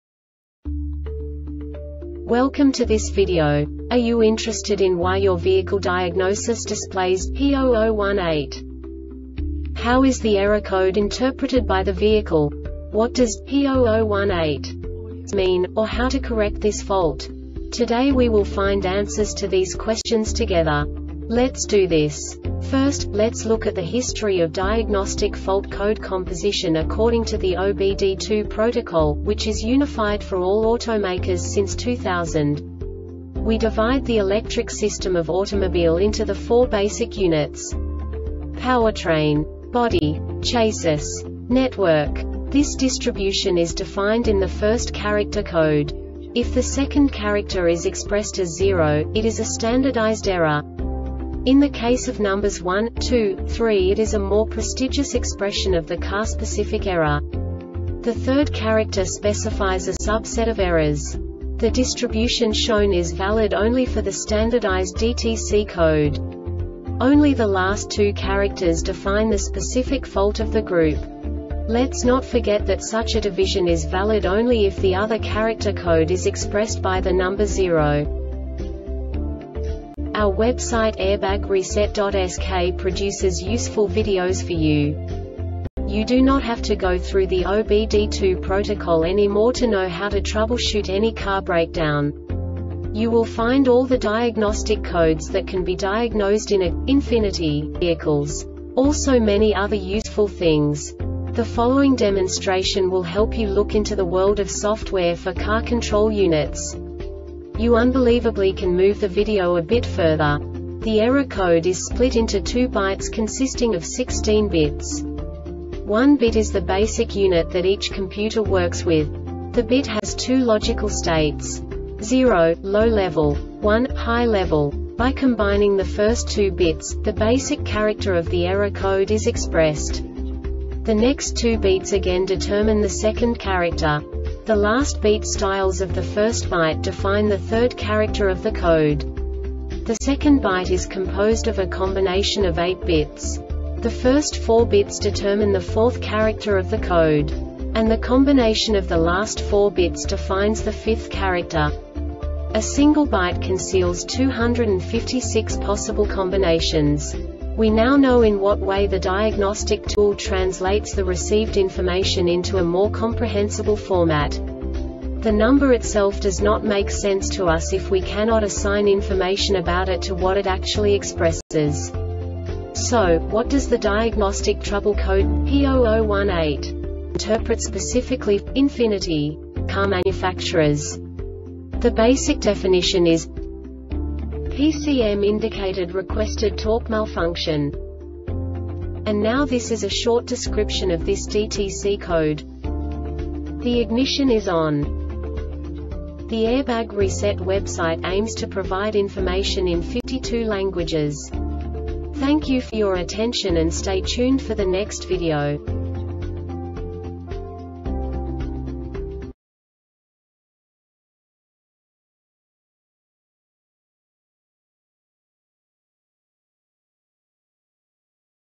DTC Ford P0018 Short Explanation
Contents:
0:21 Basic DTC analysis according to OBD2 protocol standard.
1:48 Insight into programming
2:58 A diagnostically understandable description of the Diagnostic Trouble Code
3:05 Basic error definition

Make:
Ford
Code:
P0018
Definition:
PCM Indicated Requested Torque Malfunction
Description: